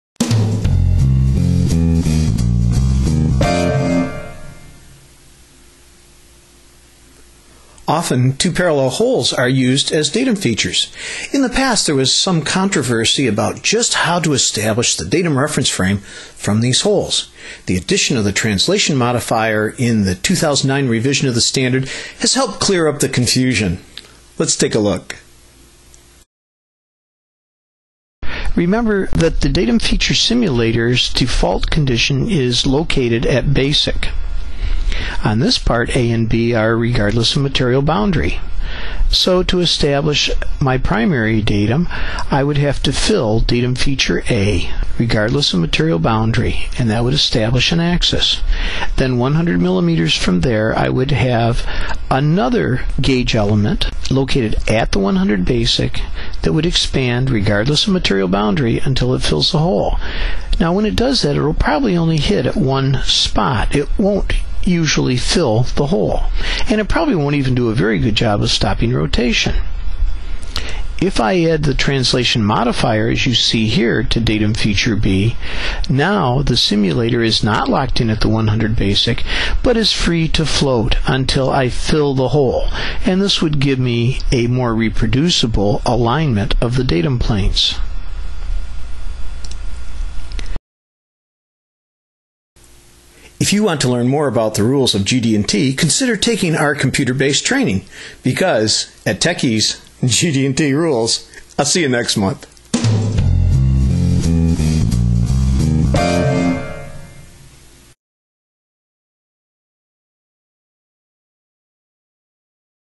GD&T Tip - Consider the Translation Modifier
GD&T Training and GD&T Materials - Don Day explains degree of freedom.